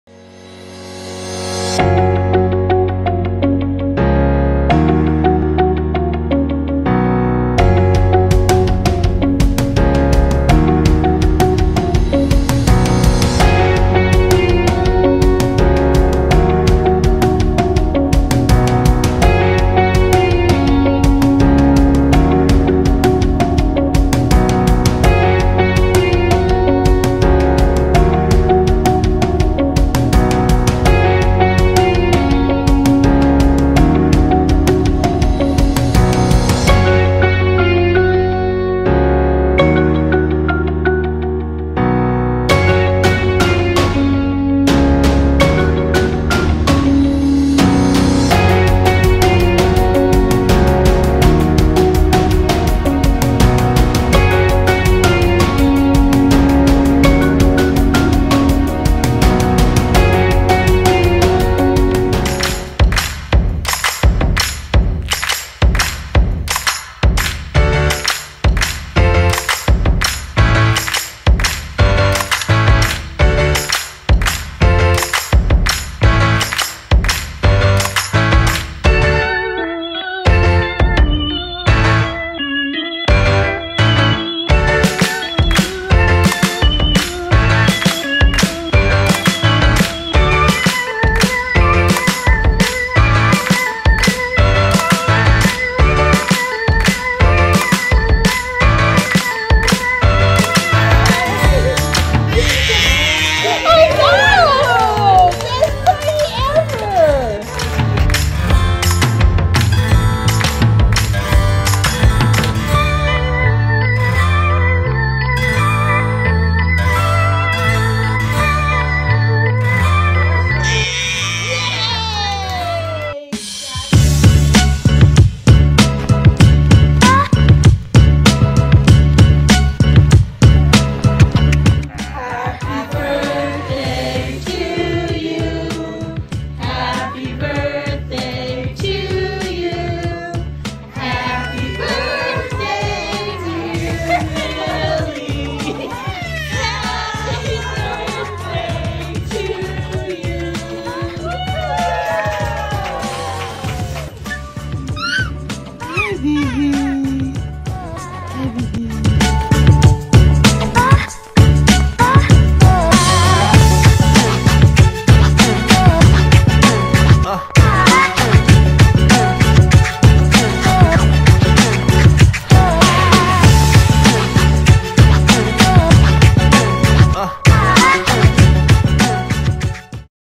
Year One! - Happy First Birthday Camille
Thank you to all our family and friends who gathered to help celebrate Camille's first birthday!